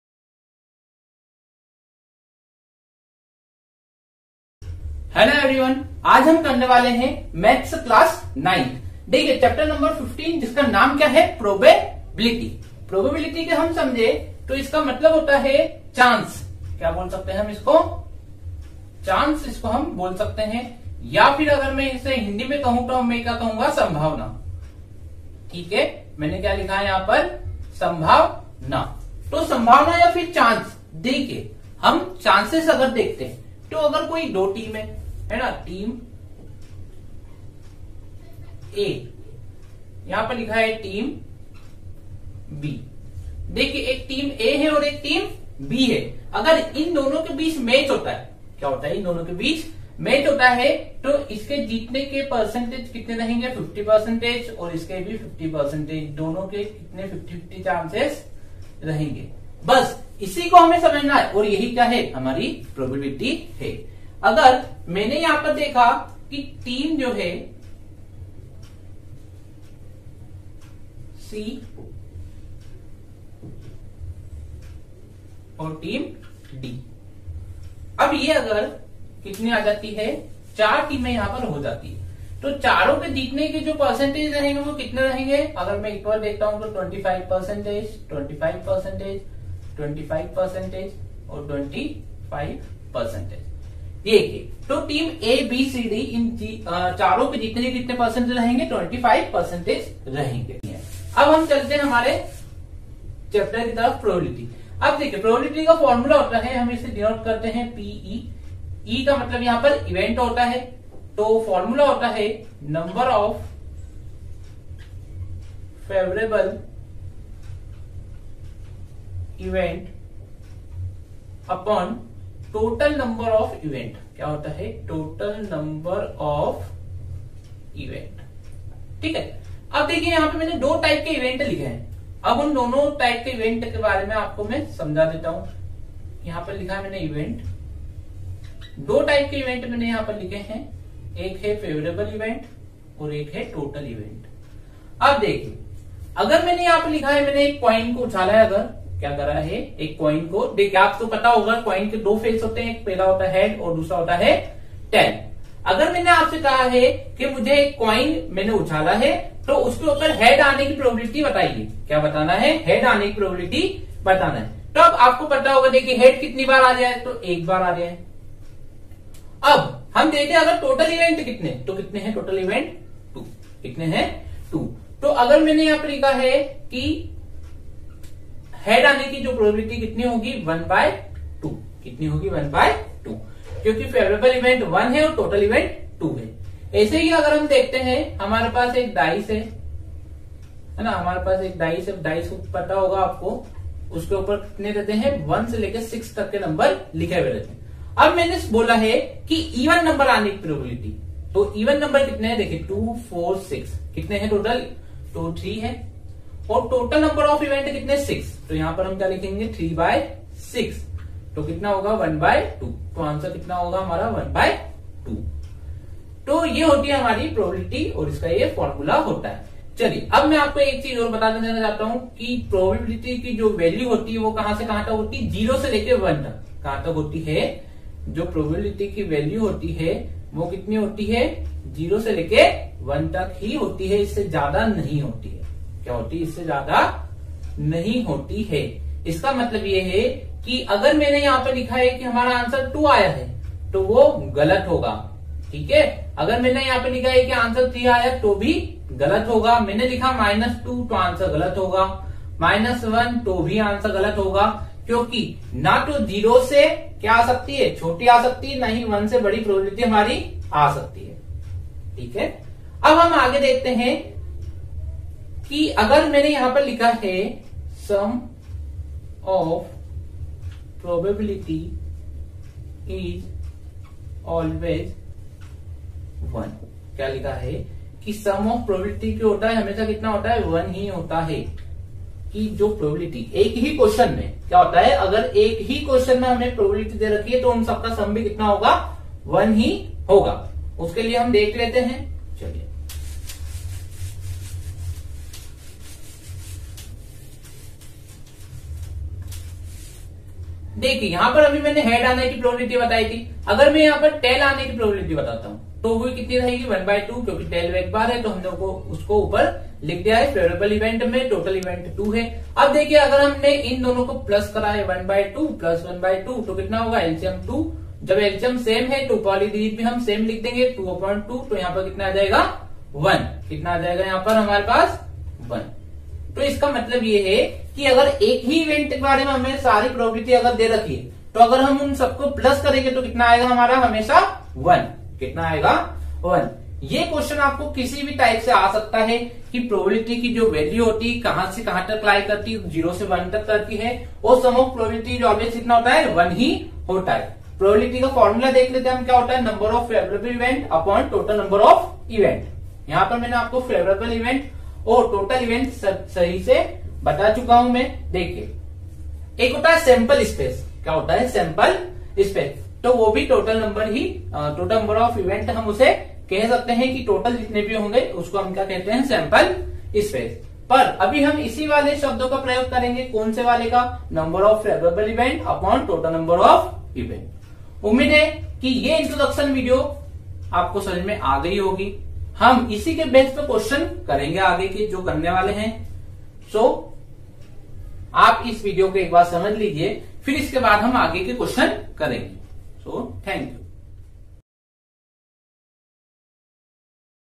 Chapter 15 Class 9th Maths (Introduction of Probability) @UVS CLASSES
Full Concept of probability with basic Knowledge.